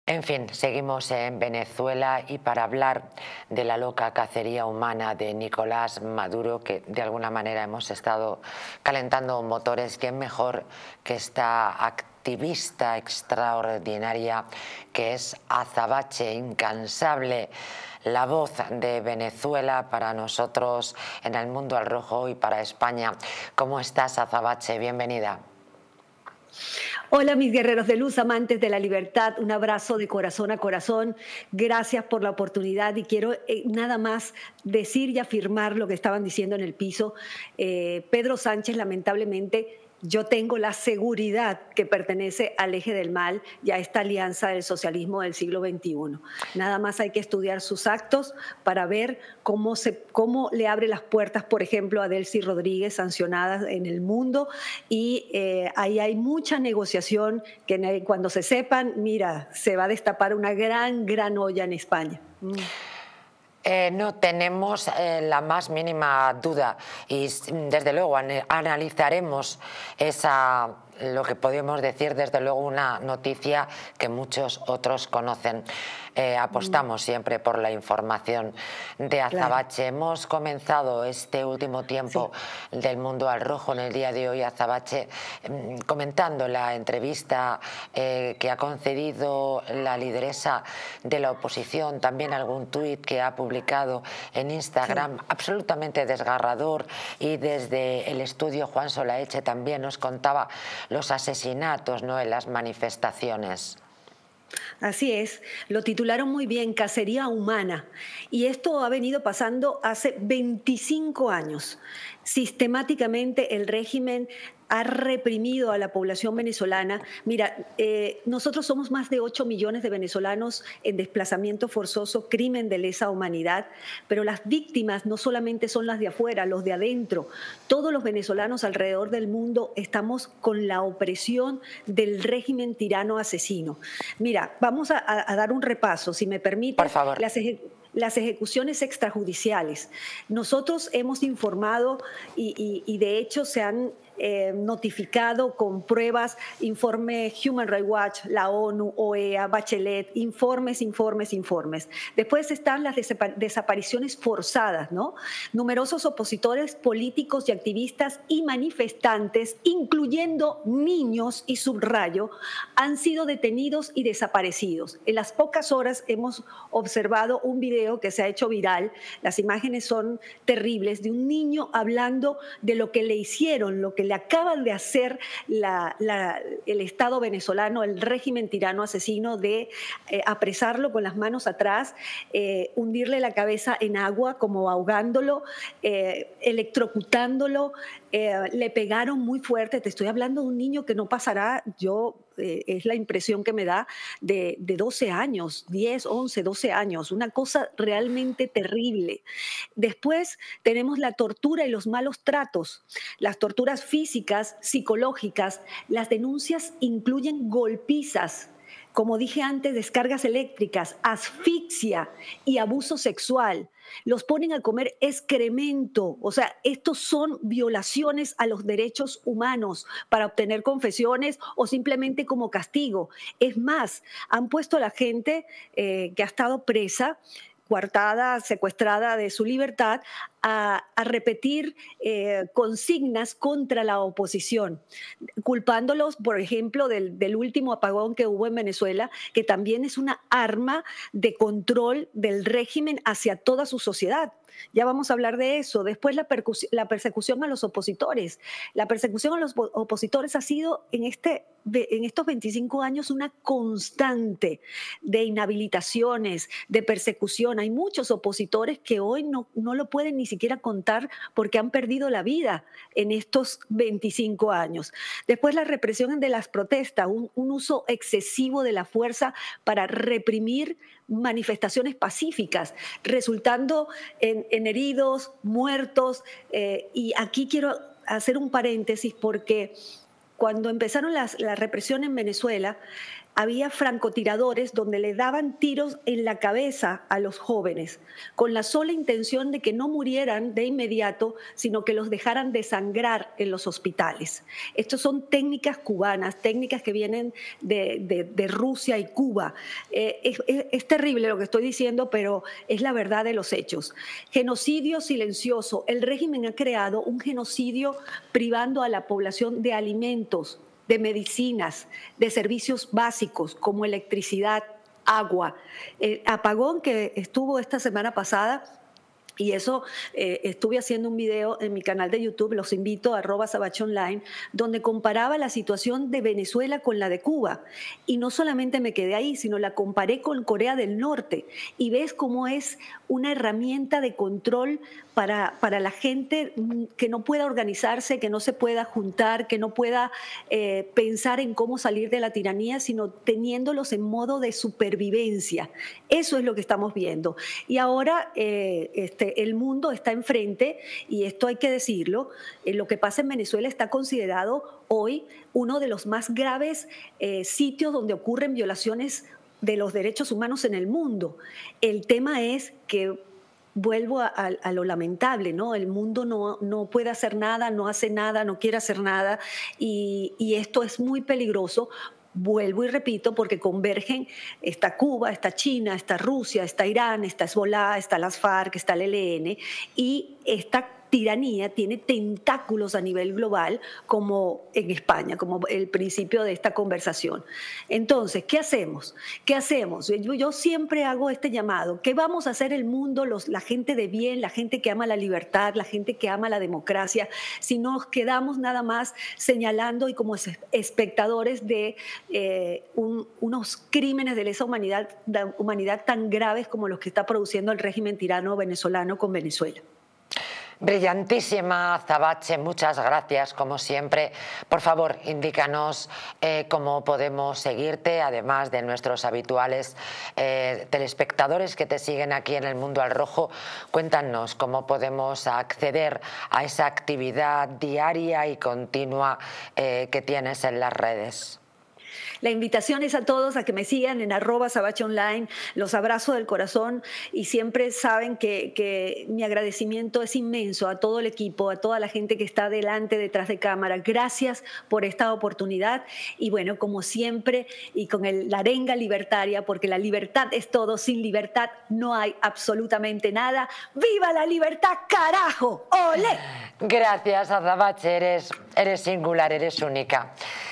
🔴VENEZUELA🔴 AZABACHE, SÁNCHEZ PERTENECE A LA AMENAZA DEL EJE DEL MAL QUE REPRESENTA MADURO🔴
En este video, hablaremos sobre la amenaza que representa Maduro y cómo Azabache, Sánchez pertenecen al eje del mal en Venezuela. ¡No te lo pierdas en DistritoTV!distritotv